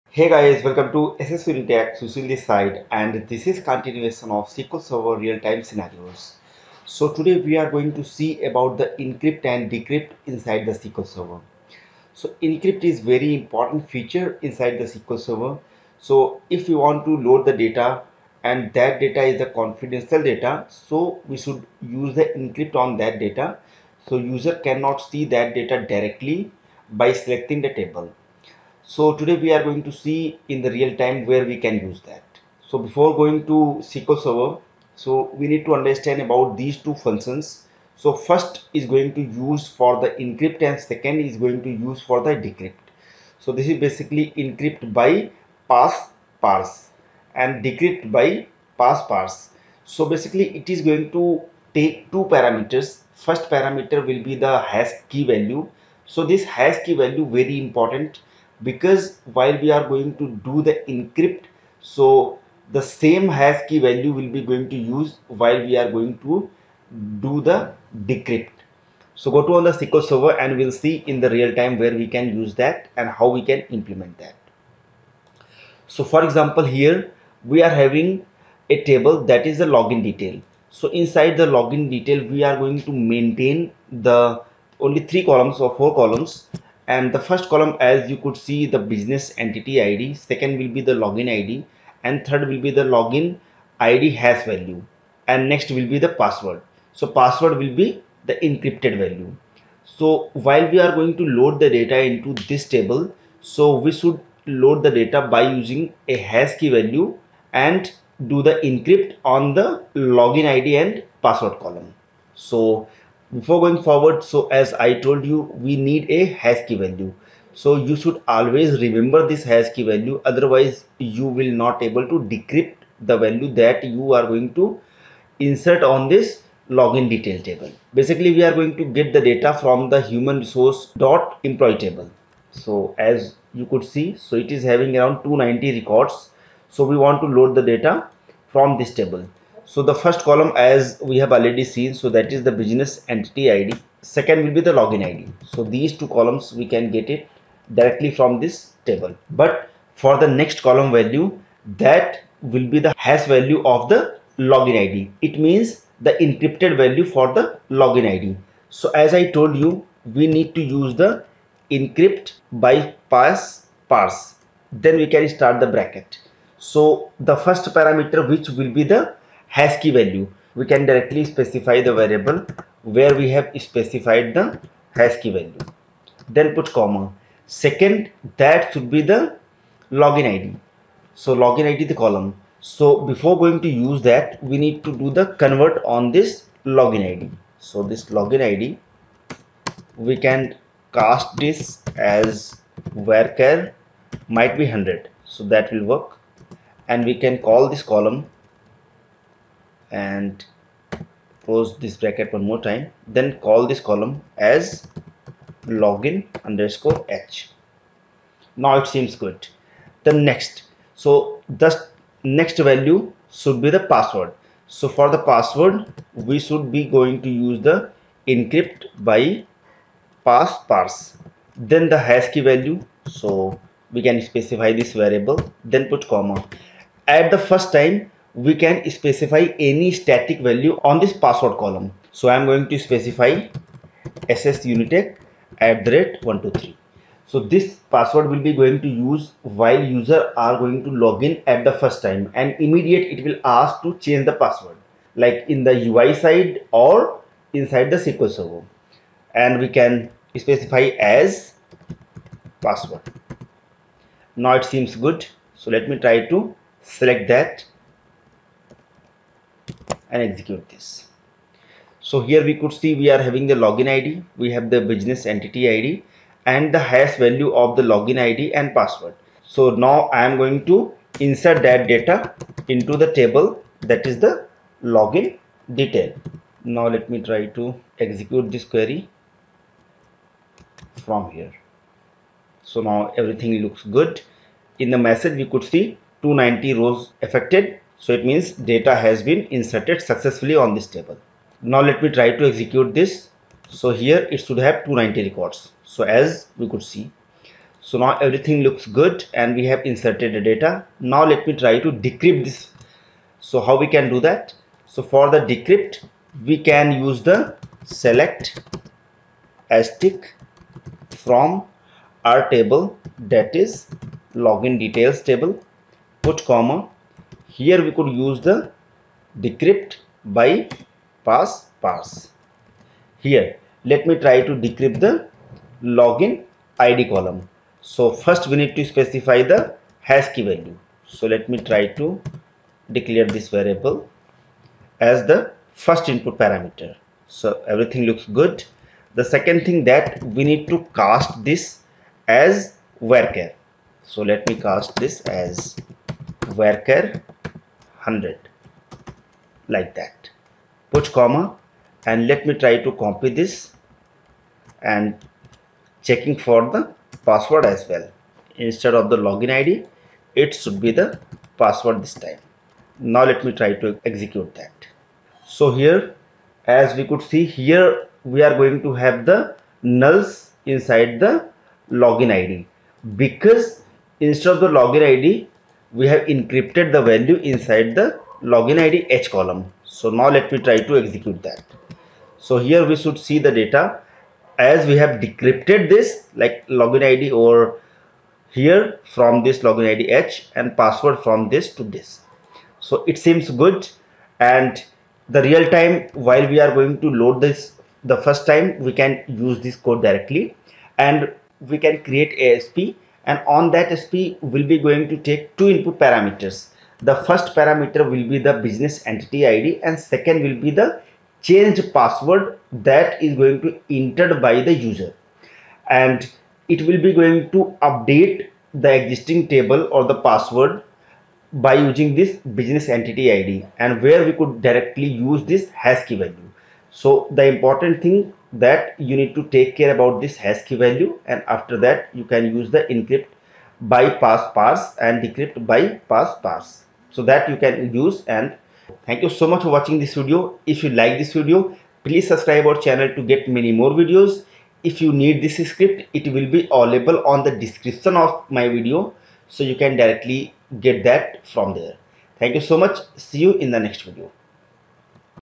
HOW TO ENCRYPT AND DECREYPT PASSWORD BY USING ENCRYPTBYPASSPHRASE AND DECRYPTBYPASSPHRASE IN MSSQL
How to encrypt and decrypt data in SQL Server
---Script
declare @hashkey varchar(100)='ssunitech'

select BusinessEntityID,
LoginID,ENCRYPTBYPASSPHRASE(@hashkey,cast(LoginID as varchar(100))) LoginID_H,
ENCRYPTBYPASSPHRASE(@hashkey,'ssunitech@123') as password
Into LoginDetails
from [HumanResources].[Employee]

select *,cast(deCRYPTBYPASSPHRASE('ssunitech',LoginID_H) as varchar(1000)) ,
         cast(deCRYPTBYPASSPHRASE('ssunitech',password) as varchar(1000))
from LoginDetails